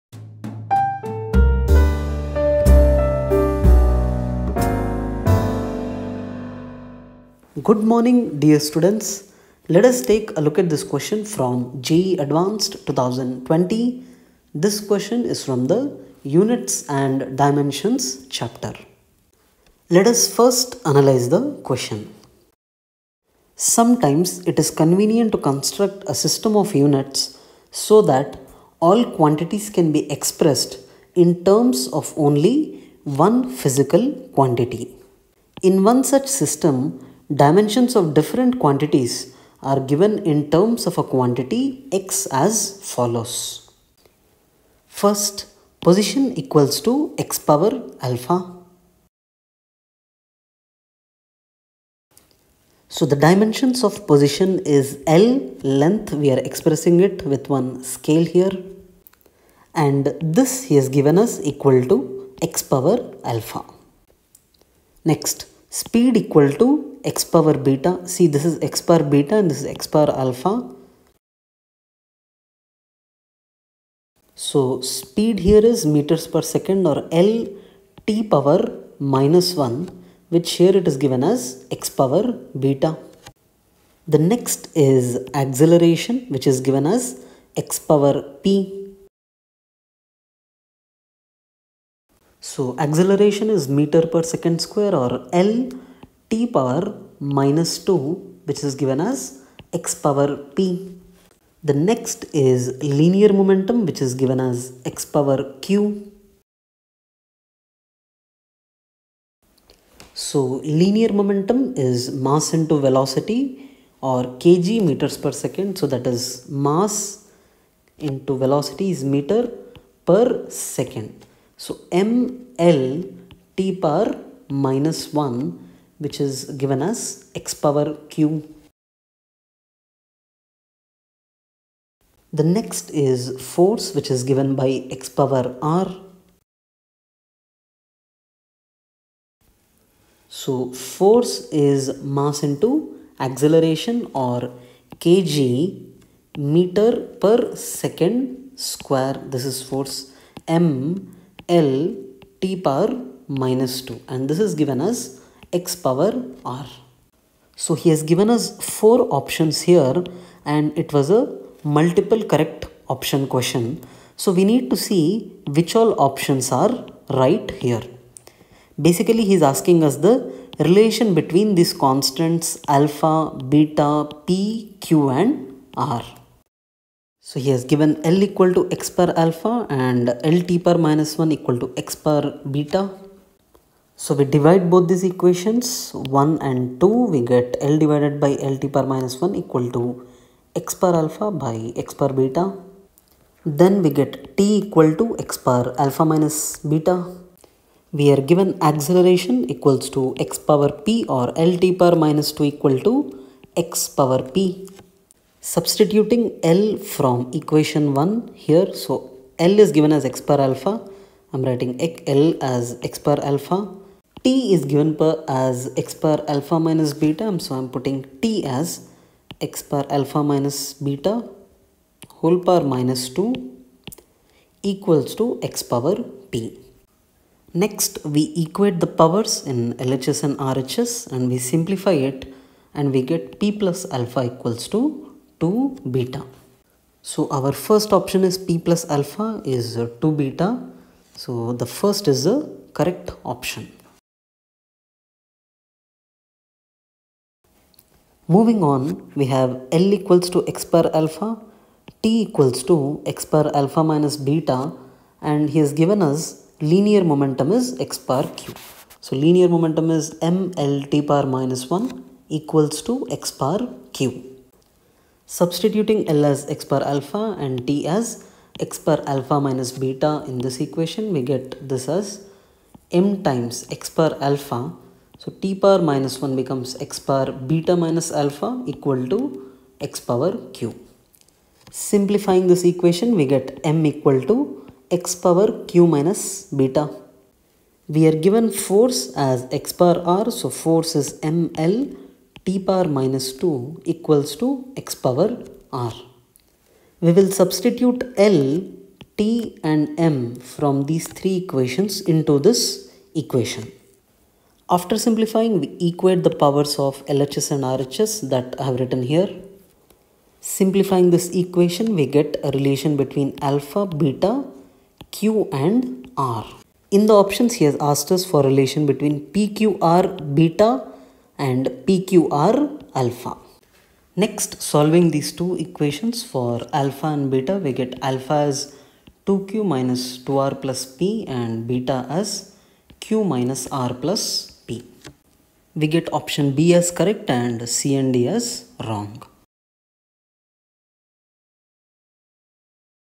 JEE Advanced 2020 solutions | Question 9 | Units and Dimensions | Class 11 | IIT JEE | Physics
0:00 Analyse Question
0:45 Visualise Question
2:55 Finding relations

Sometimes it is convenient to construct a system of units so that all quantities can be expressed in terms of only one physical quantity. In one such system, dimensions of different quantities are given in terms of a quantity X as follows: [position] = [𝑋 𝛼];    [speed] = [𝑋𝛽];     [acceleration] =[𝑋p];     [linear momentum] = [𝑋q];     [force] = [𝑋r].